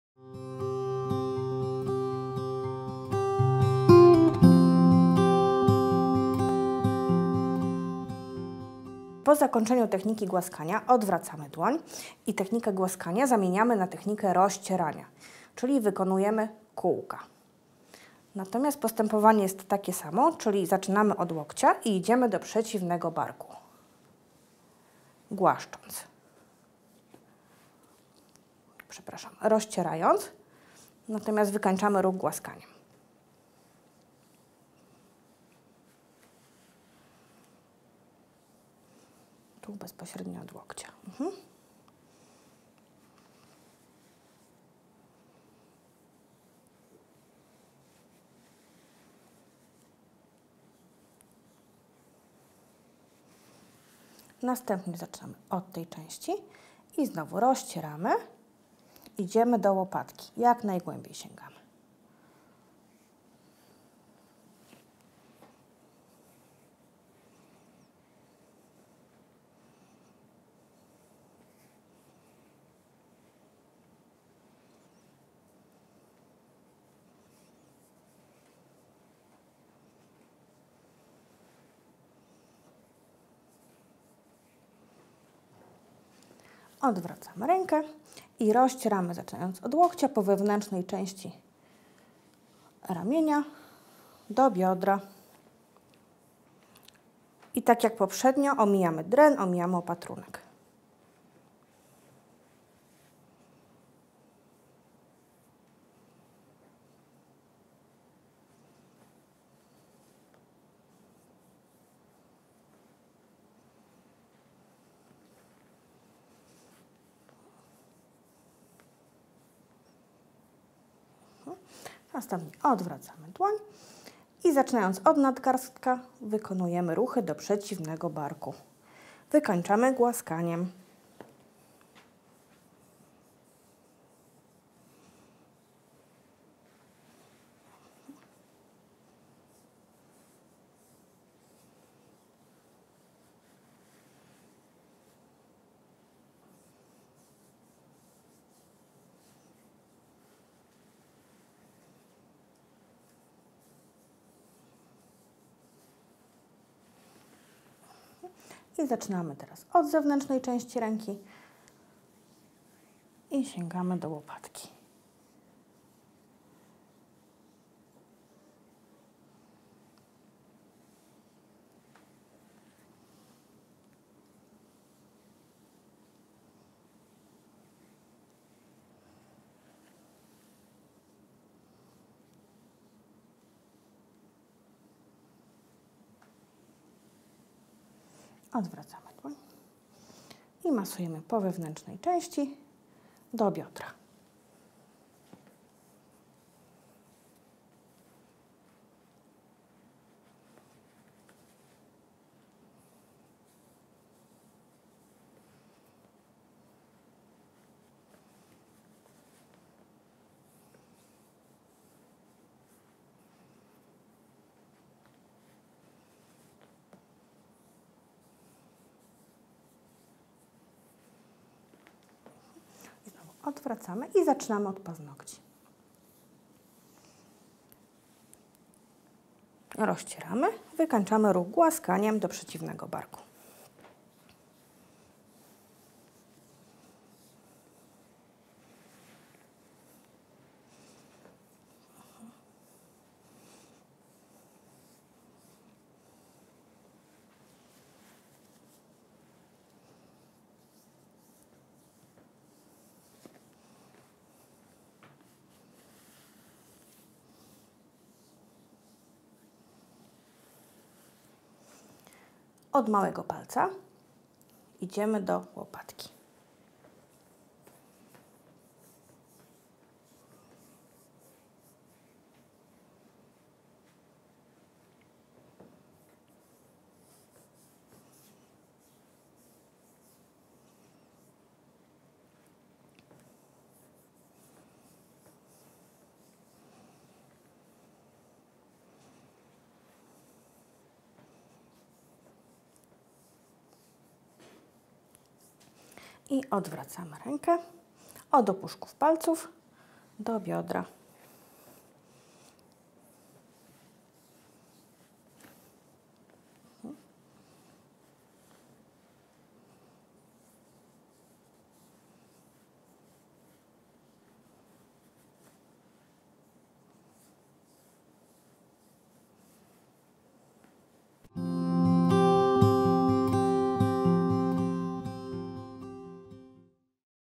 Ćwiczenia, które możesz wykonywać sama – spróbuj! Nauka automasażu cz. 2 – technika rozcierania.
Film instruktażowy pokazujący technikę rozcierania automasażu. Dowiedz się więcej na